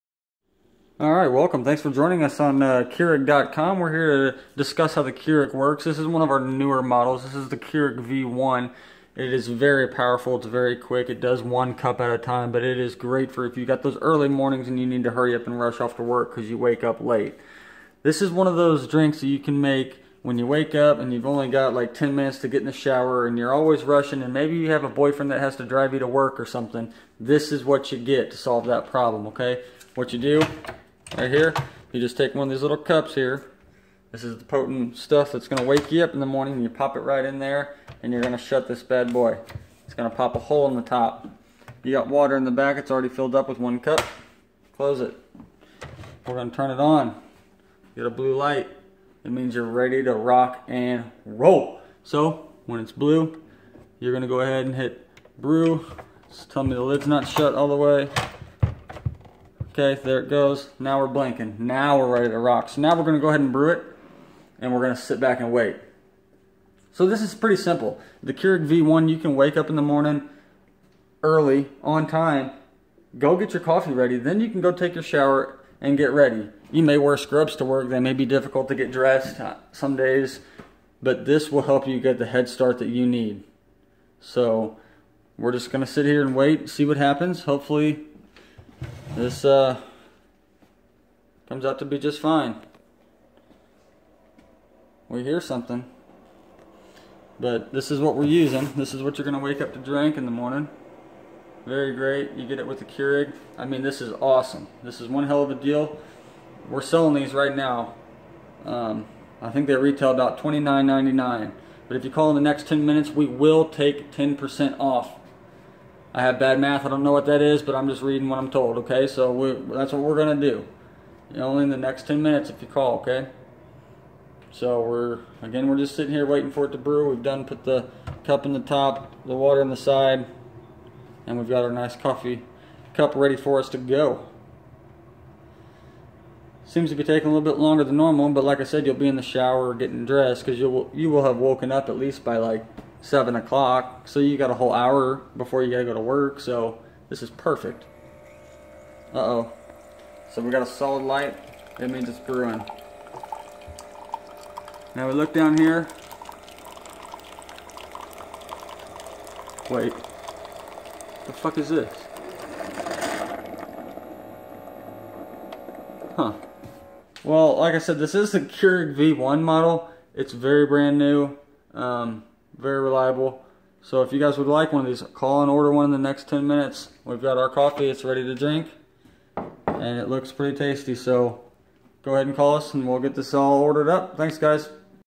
Keurig mini coffee pot review
This is my review on the keurig mini. Enjoy. They are amazing! I love mine!!